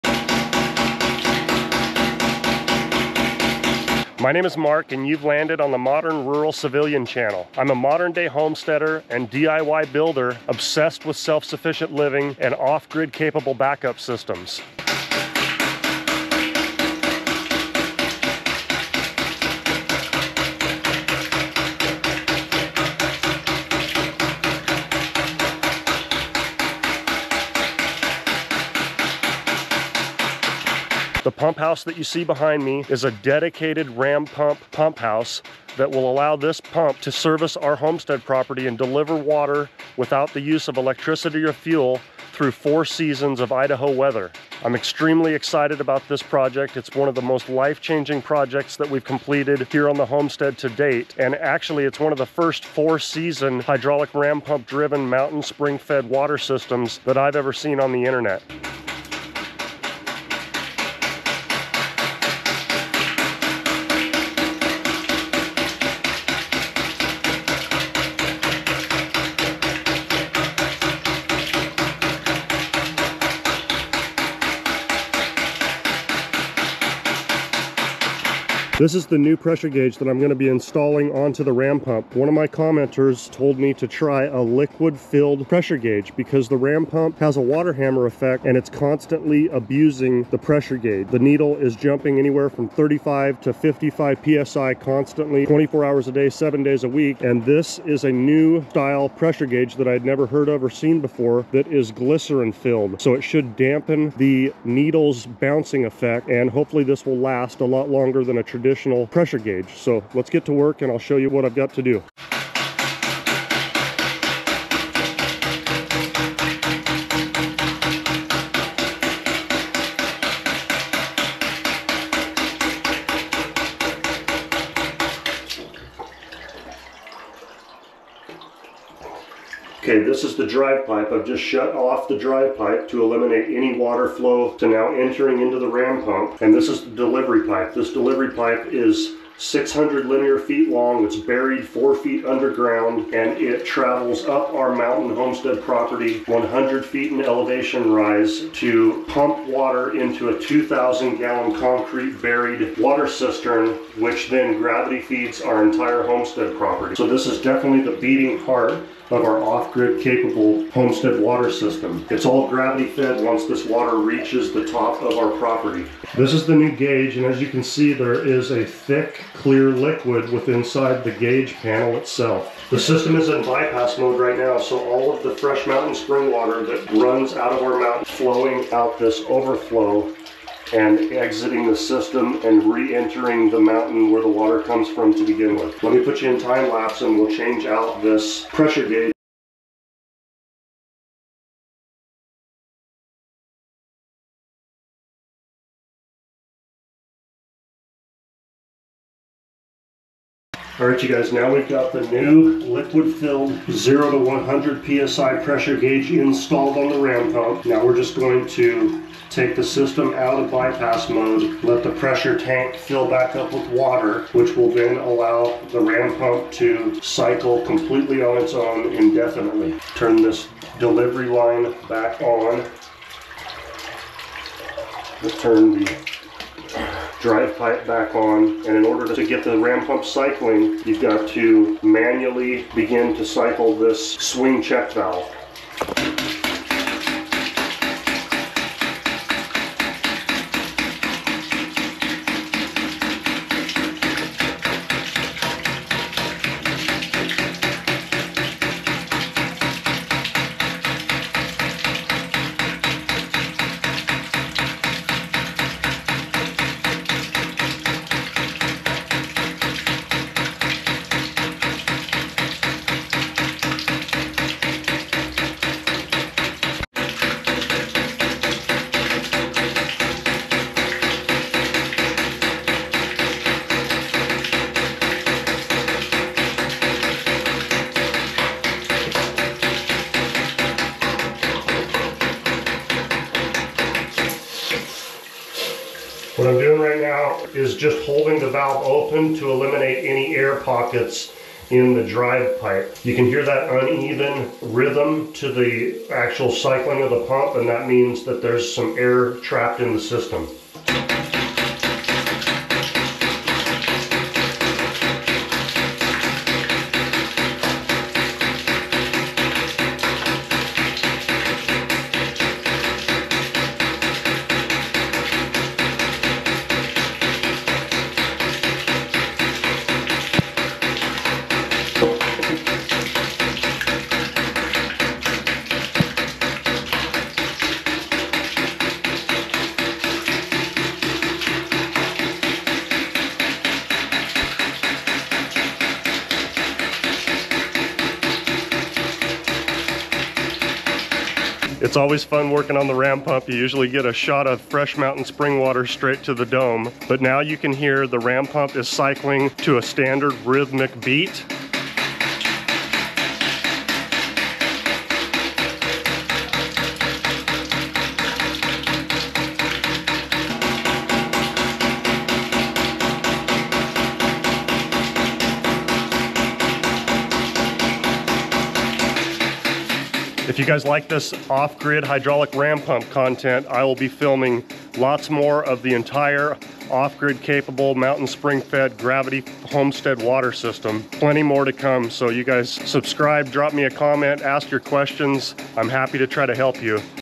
DIY Ram Pump Powered Water System
Mountain spring fed, DIY, ram pump powered, off-grid homestead water system. Today we are upgrading the standard  pressure gauge on our ram pump to a liquid filled gauge. This off-grid water system is designed to be a 4 season capable ram pump powered homestead water system. The ram pump has a dedicated pump house to insulate it from the Idaho winters as well as carry the expelled water away from the pump house and return it to the mountain.